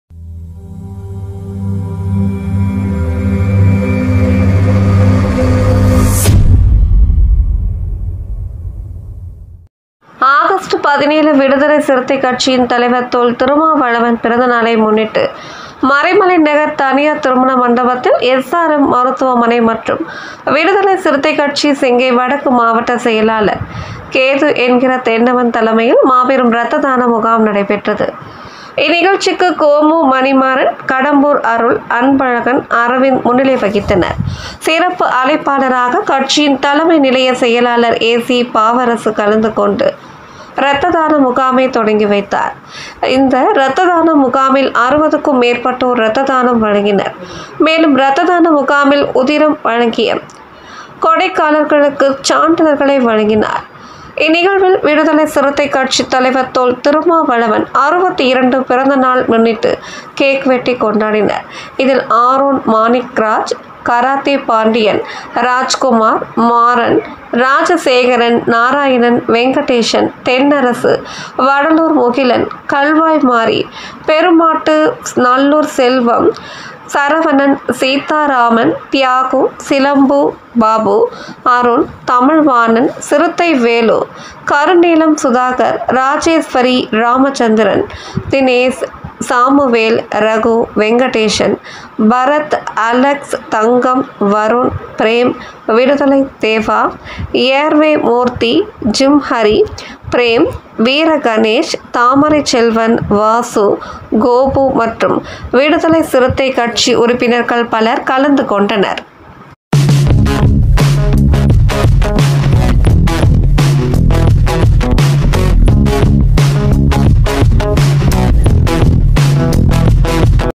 தொல்.திருமாவளவன் பிறந்தநாளை முன்னிட்டு நடைபெற்ற ரத்ததான முகாம்.
விடுதலை சிறுத்தைகள் கட்சி தலைவர் தொல்.திருமாவளவன் MP பிறந்தநாளை முன்னிட்டு செங்கை வடக்கு மாவட்டம் சார்பில் நடைபெற்ற ரத்ததான முகாம்.